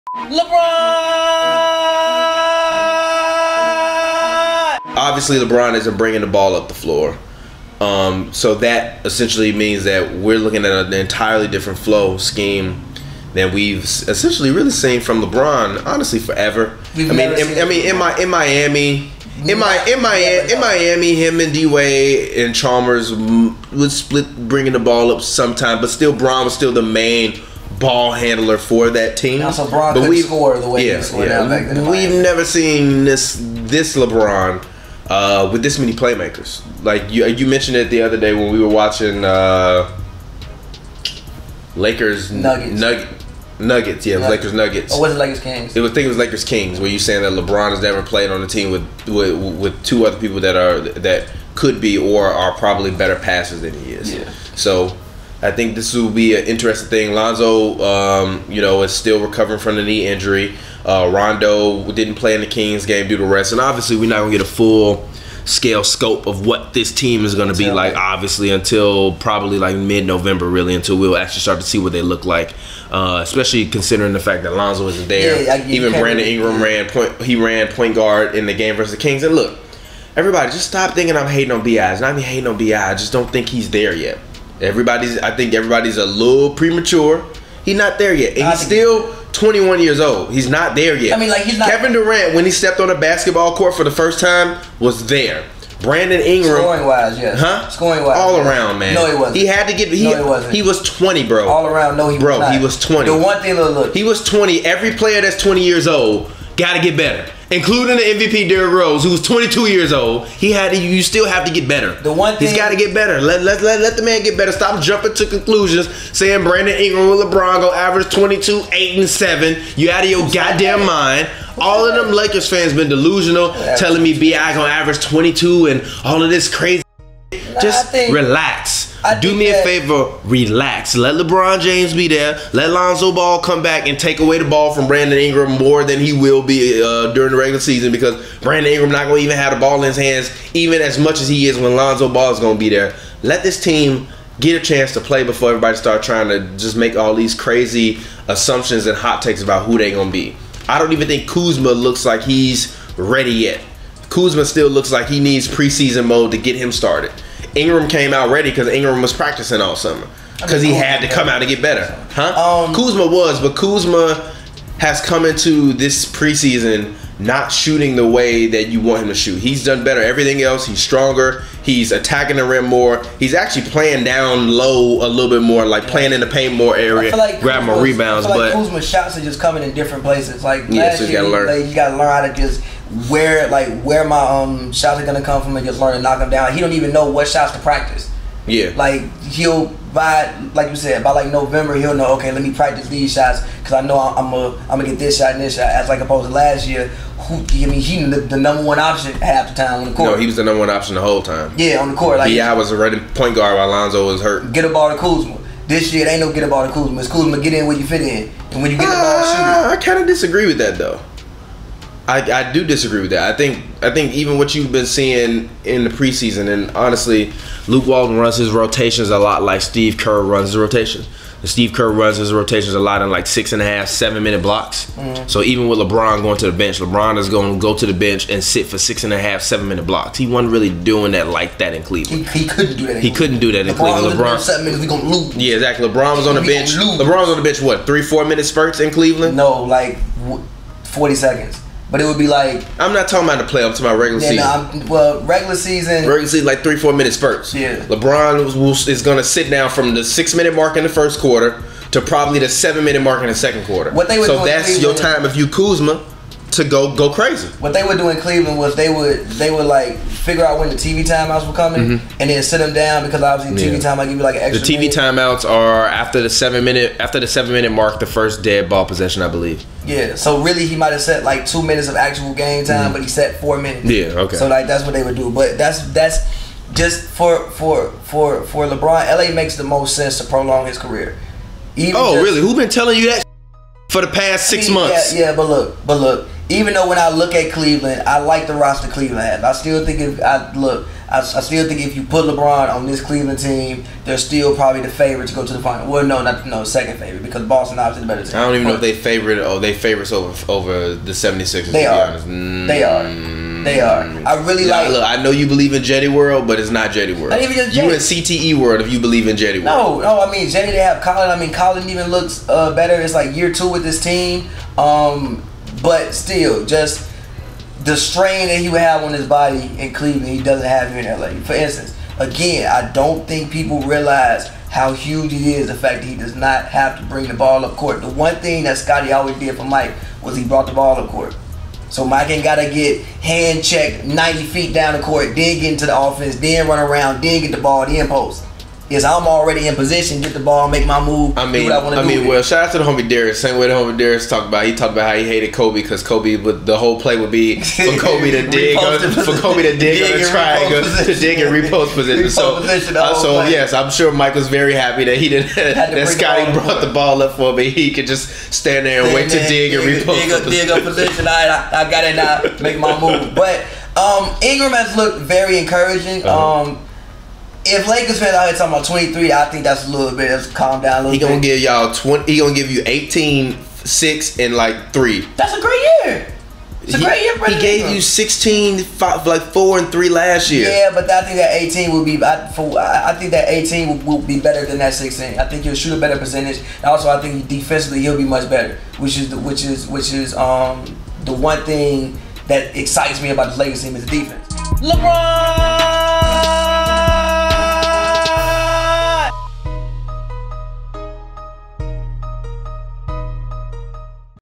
Are Lebron's New Lakers Teammates Fitting? | KingWatch | Hoops N Brews
🟢 TIP JAR/DONATIONS or $HNBMedia Pavy (@Pavyworld) & TPJ (@ReelTPJ) discuss if Lebron's new teammates are a good fit with Lebron.